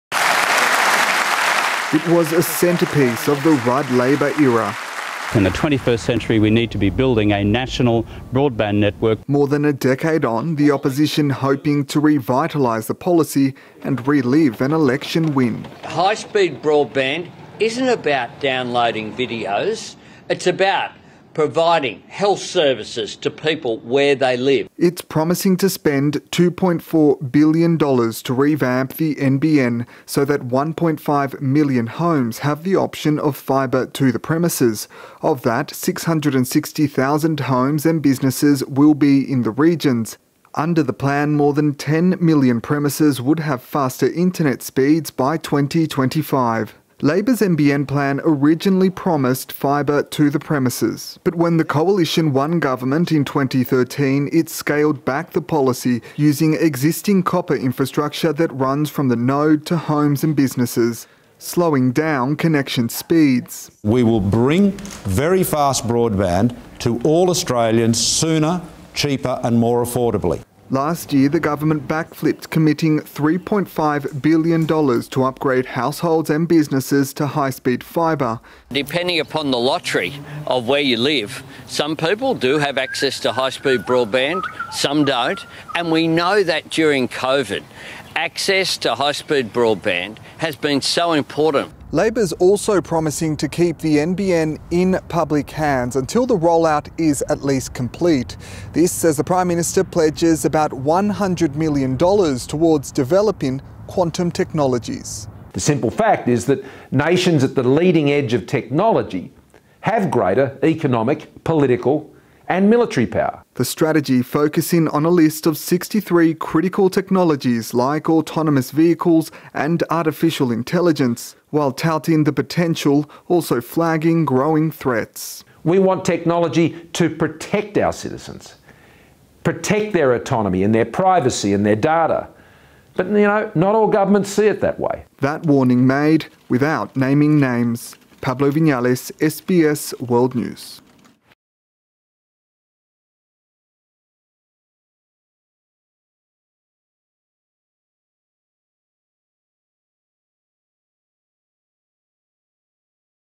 Spending promises announced as election moves into spotlight | SBS News
Both major parties announced big spending promises today as speculation builds over the timing of the next election.

The government is pledging a massive boost in spending to develop quantum technologies seen as vital in defence and national security.

While the opposition is promising high-speed fibre internet to more homes, if elected.